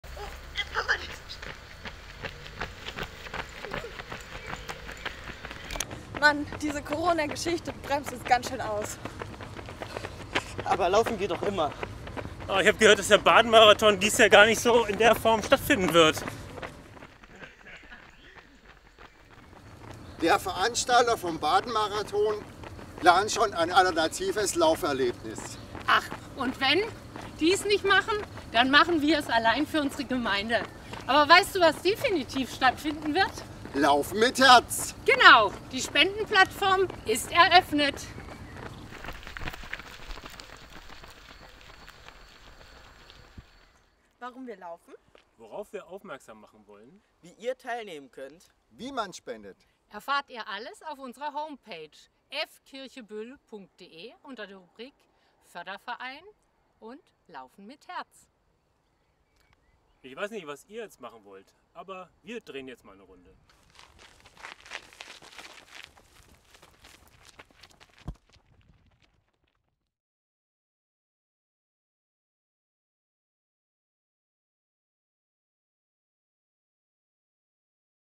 Laufen mit Herz
Mach mit, unterstütze unser Läuferteam „Laufen für Bühler Kinder“ mit deiner Spende!
Mach mit, und werde Dauerspender, so dass unser aller Projekt finanziell auf stabilen Füßen steht!
Mach mit, schnüre deine Laufschuhe und lauf mit uns !!!

Unsere Gemeinde hat es sich als Ziel gesetzt, Kinder und ihre Familien zu stärken und zu stützen. Als Kernstadt-Kirche mit großem Einzugsgebiet sehen wir es als unseren Auftrag, als Gemeinde zu wirken. Unsere Kinder- und Familiendiakonin Ute Gatz setzt sich hier mit viel Herz und Engagement ein. Die Finanzierung ihrer Stelle ist unser aller stärkstes Projekt.

Eine Produktion von Jonas Borho